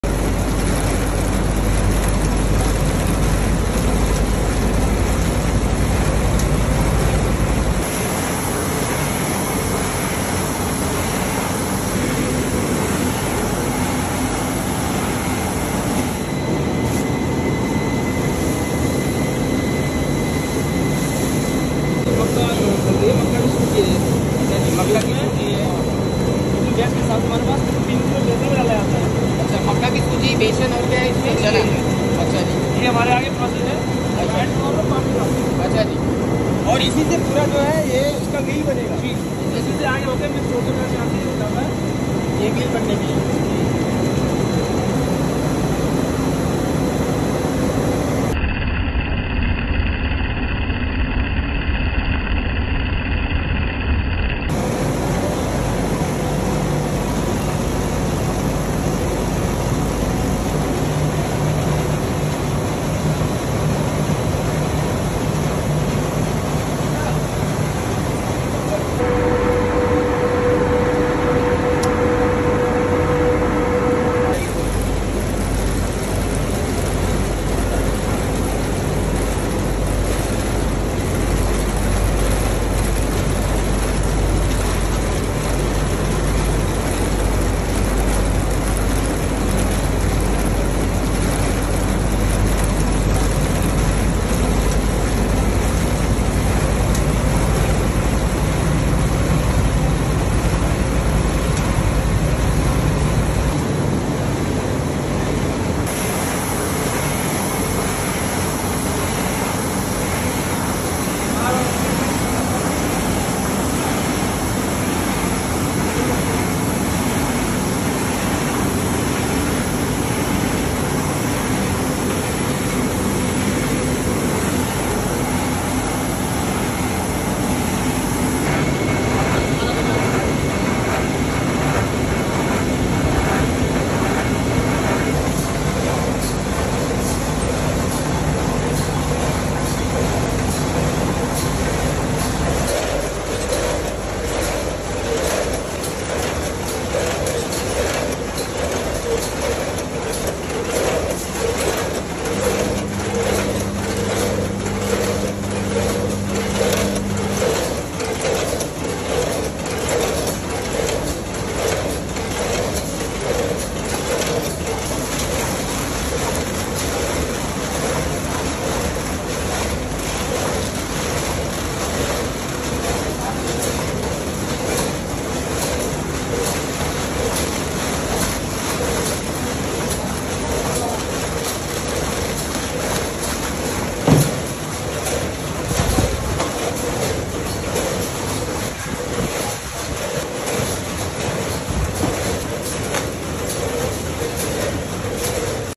देखिए Factory में कैसे बनती हैं Motu Patlu Rings | Factory Food | Indian Street Food
बच्चों की मनपसंद मोटू पतलू रिंग को फैक्ट्री में कैसे बनाते हैं

Hathras Uttar Pradesh